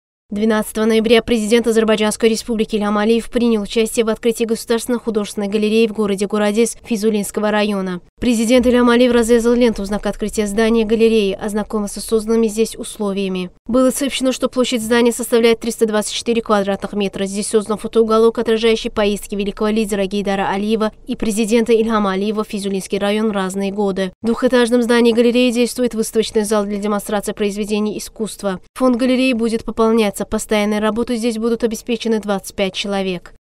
Президент Ильхам Алиев принял участие в открытии художественной галереи в городе Горадиз
Президент Ильхам Алиев принял участие в открытии Государственной художественной галереи в городе Горадиз